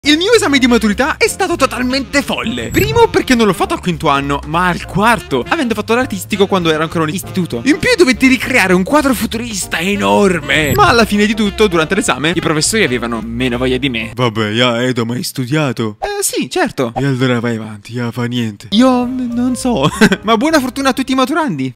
"VABE JA FA NIENT" 😅
Il mio esame di maturità è stato totalmente folle, anche perché i professori avevano meno voglia di me.